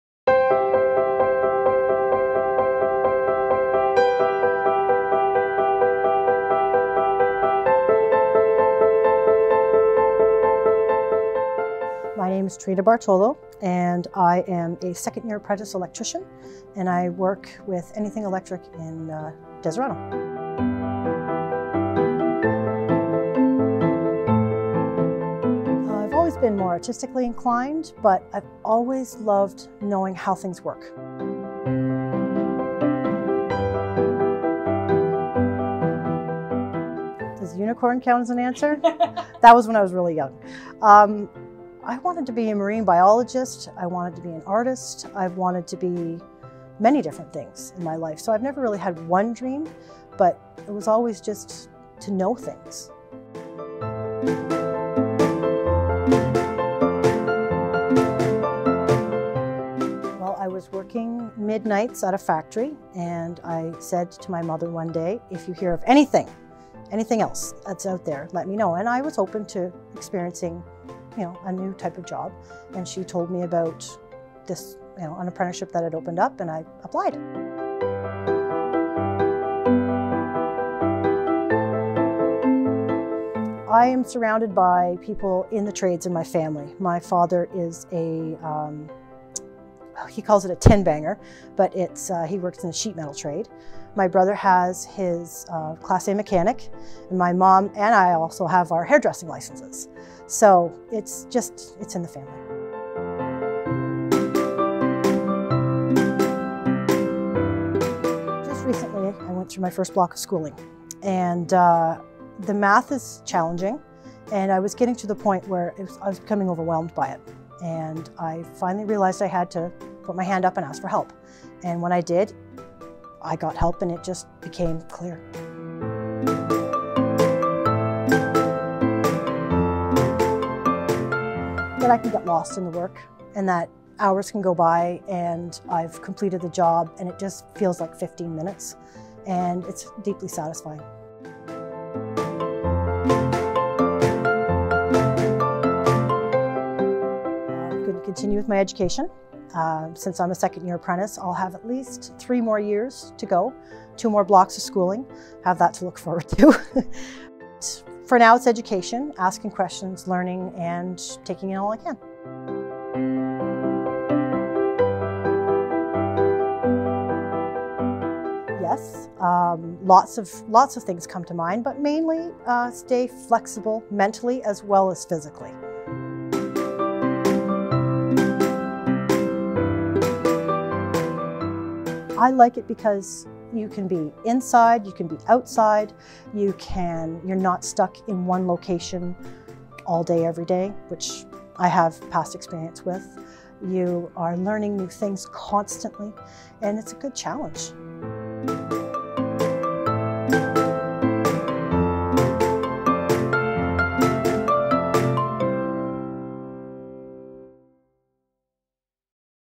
LDSB 2018 Women in Trades: Electrician
The Limestone District School Board hosted more than 400 female students in Grades 7 and 8 at a Women In Trades day held on October 24, 2018 at St. Lawrence College. The day was made possible through funding from the Ontario Youth Apprenticeship Program and provided student participants with the opportunity to interact with and ask questions of women working in the skilled trades, and attend a trade show featuring different occupations involving the skilled trades.

Electrician Trina Bartolo was one of the featured presenters.